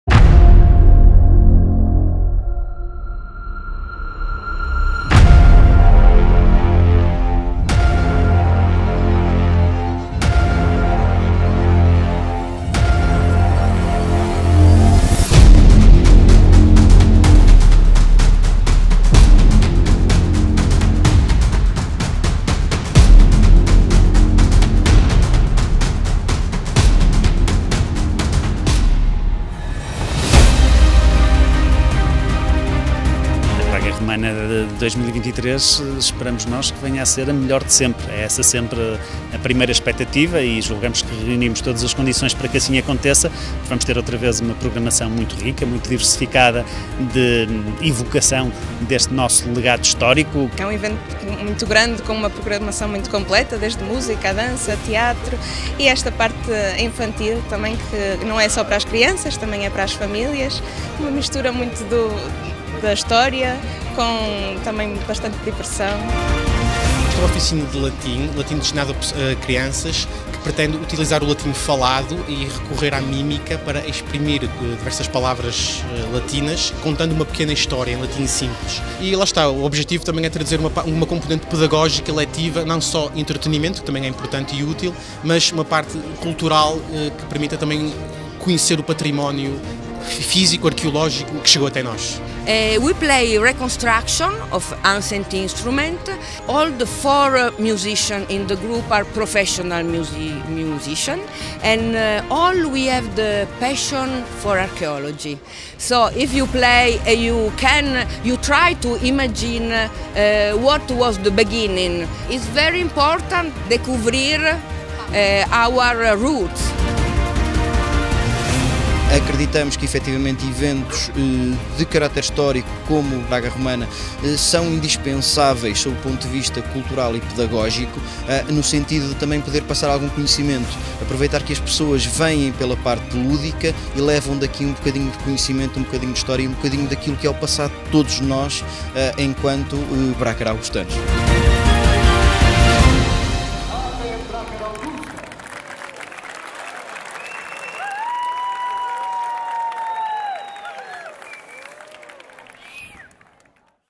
Braga Romana 2023
Os Deuses alinharam-se favorecendo o sucesso da edição 2023 da Braga Romana - Reviver Bracara Augusta.

De 17 a 21 de Maio, foram cinco dias protegidos pelos bons desígnios de Flora, uma verdadeira força da natureza entre os Romanos, que trouxe o sol, as flores e os aromas primaveris.

As ruas de Bracara Augusta cobriram-se de alegria, pedagogia e festa!

Romanos, brácaros, galaicos e muitos milhares de visitantes celebraram em paz e com entusiasmo a Opulenta e Aeterna Bracara Augusta.

Até para o ano!